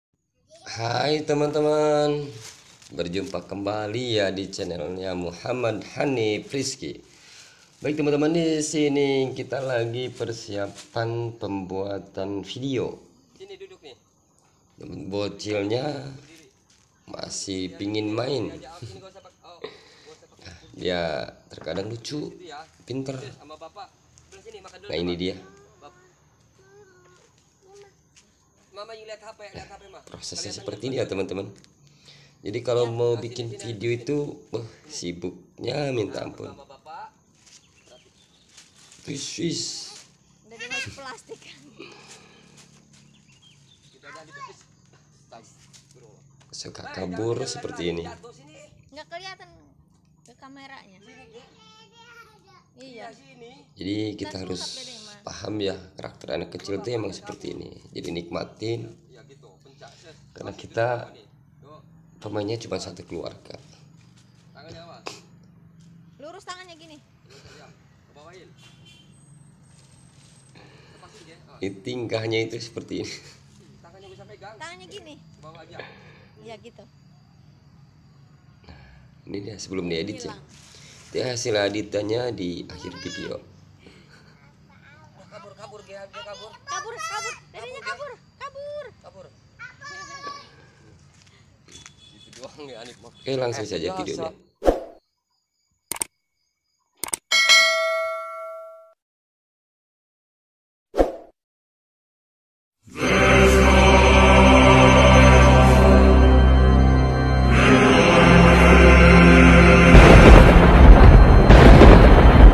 Di balik Layar Proses Pembuatan Vidio Sulap Editing Vidio _ Vidio Menghilang
menghilang, jadi ular, sulap, mejikom